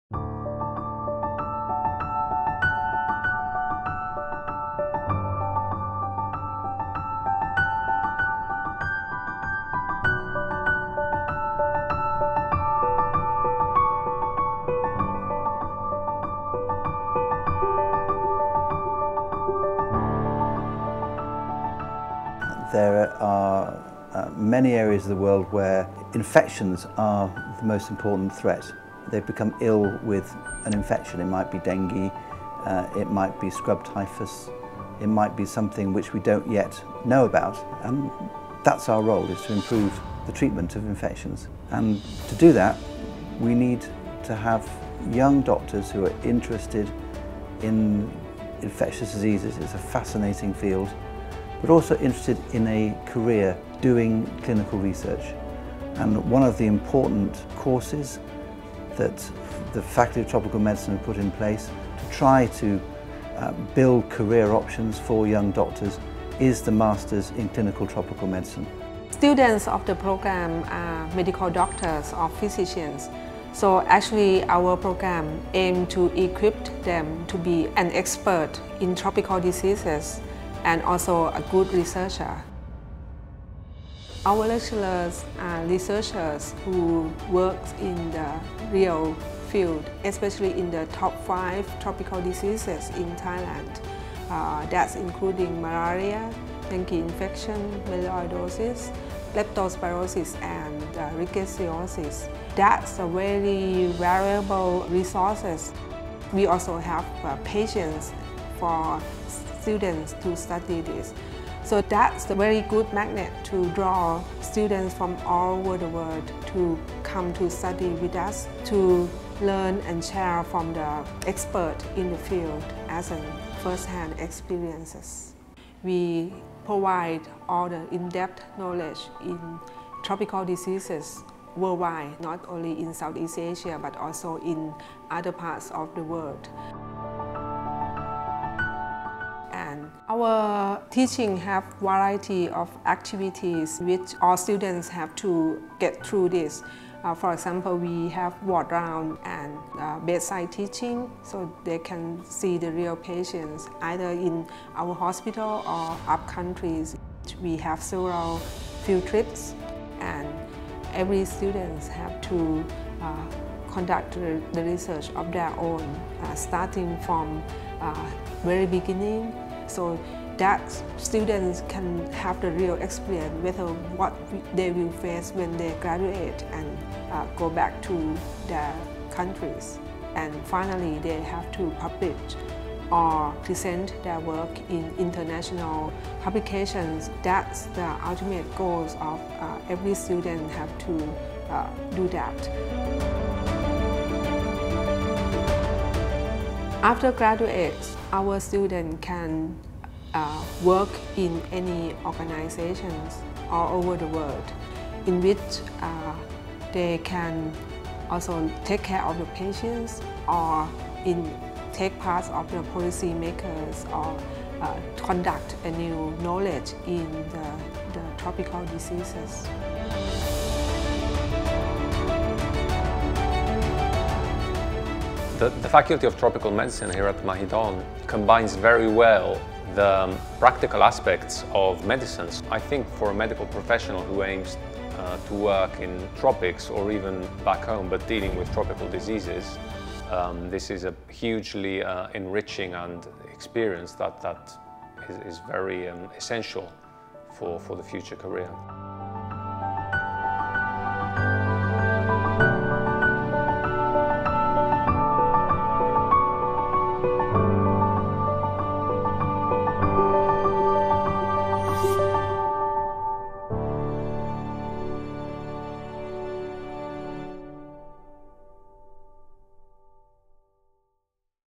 Master of Clinical Tropical Medicine International Program, Faculty of Tropical Medicine
136th AUN-QA Assessment at Programme Level
Master of Clinical Tropical Medicine International Program
Faculty of Tropical Medicine,  Mahidol University, Thailand